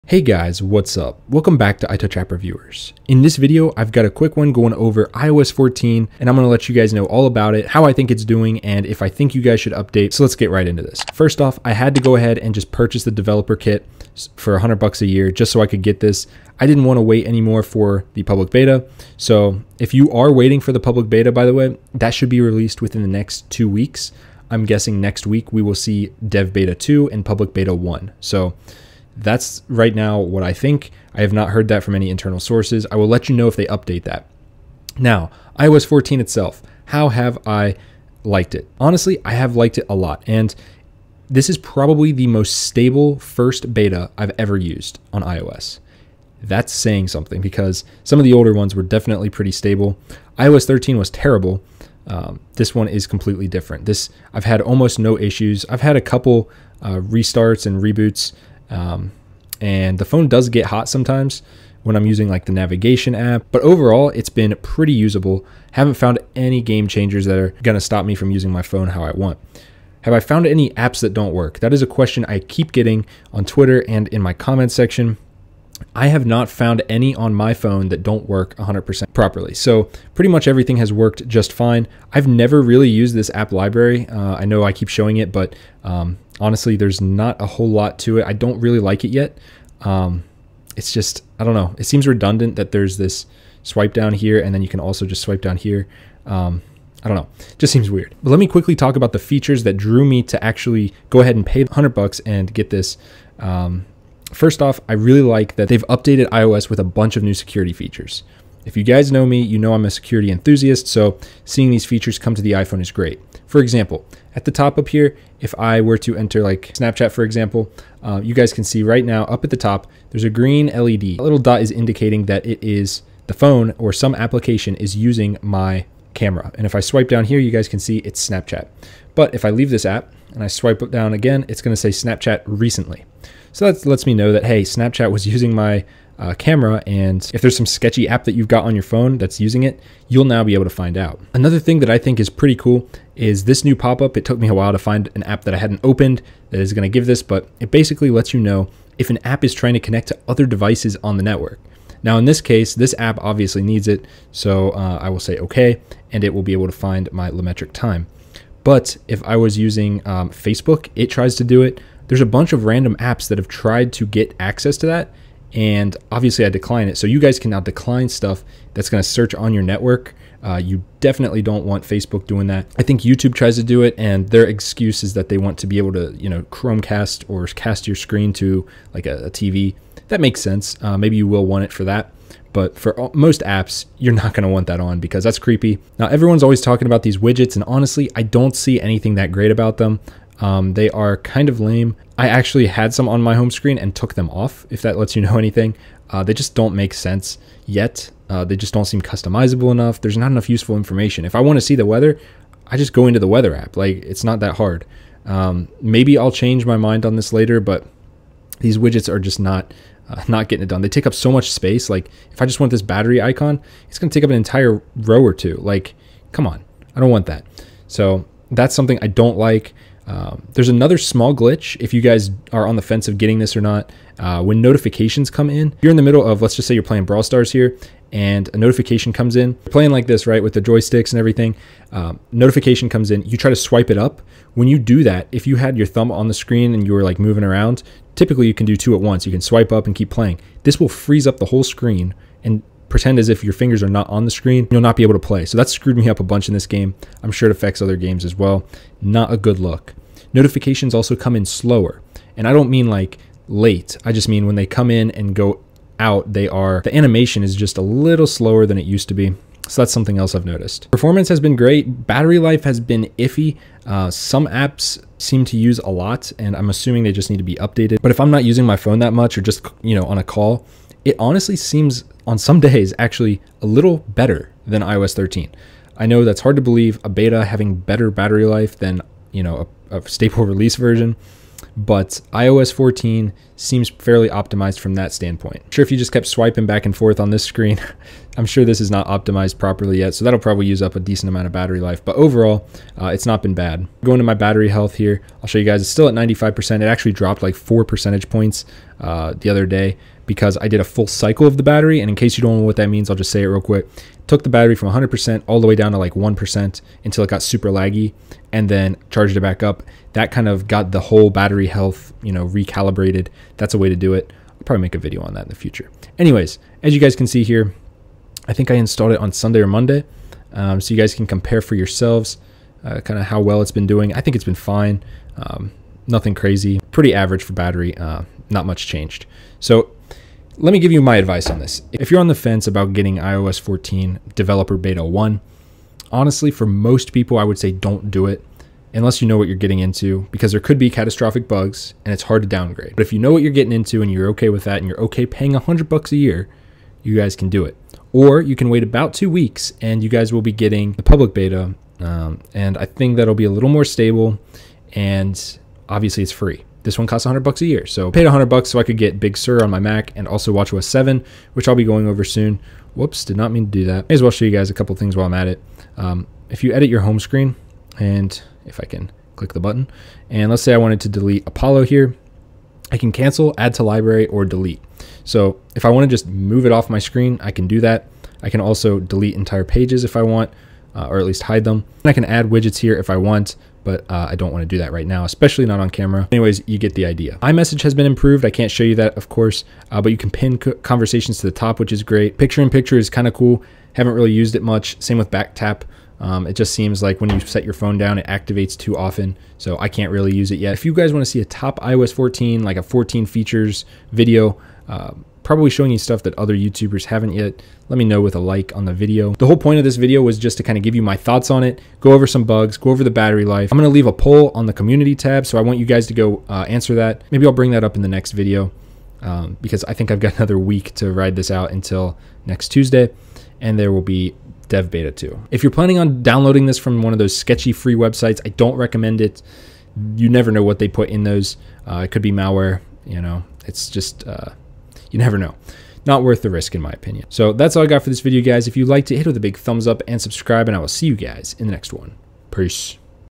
iOS 14 // Should You Install the Public Beta? // Features + Privacy Features (NEW)
iOS 14 has been out for over a week now and I have Developer Beta 1 on my iPhone 11 Pro. iOS 14 brings brand new features like App Library, Widgets, privacy and security enhancements, and more! Want to know when the iOS 14 Public Beta launches? We have all of the release dates for iOS 14 here.